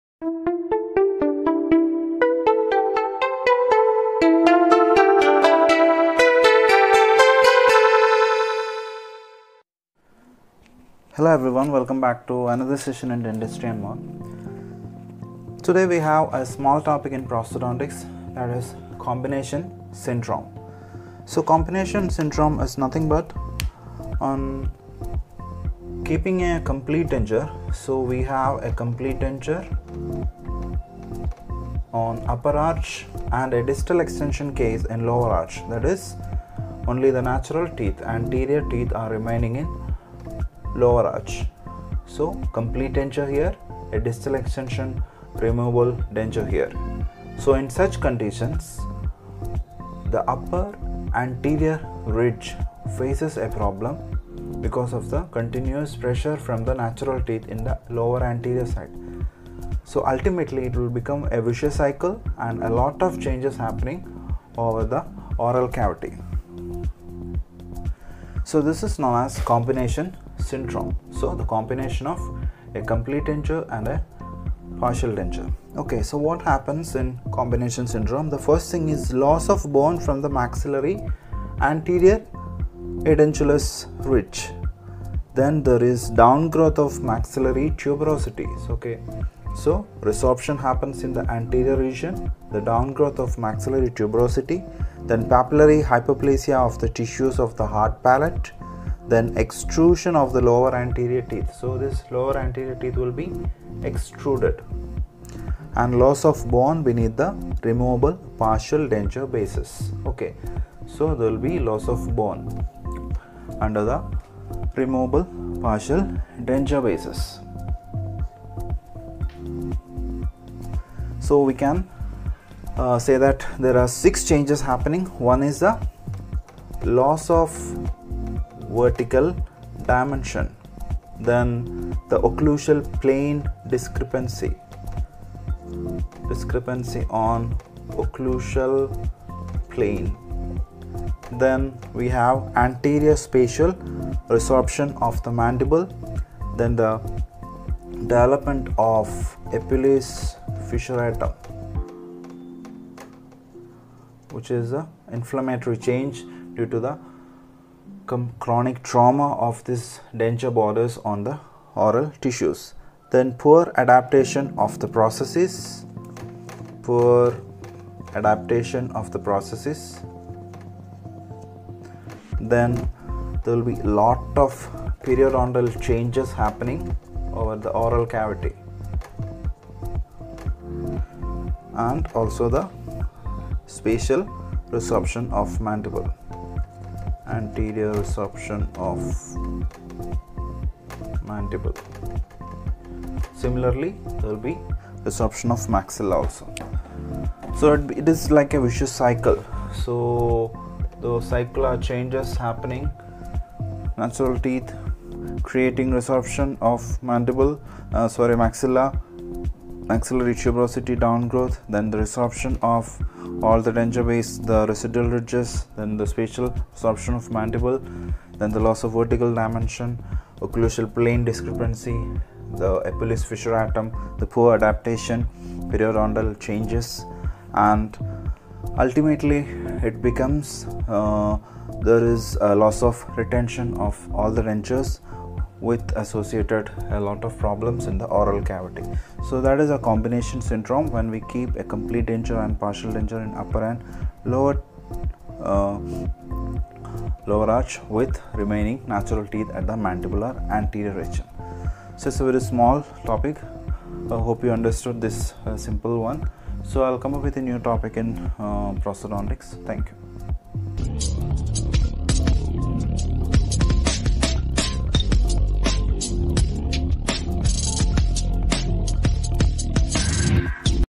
COMBINATION SYNDROME
Combination syndrome (CS) is defined as “a condition caused by the presence of the lower anterior teeth and the absence of the posteriors and resulting in significant maxillary anterior alveolar resorption.”1 This condition often develops in cases of a complete maxillary denture opposing a bilateral distal extension

Mic used

2)Oral medicine & Radilology

3)Oral pathology

4)Pedodontics

5)Orthodontics

6)Public health dentistry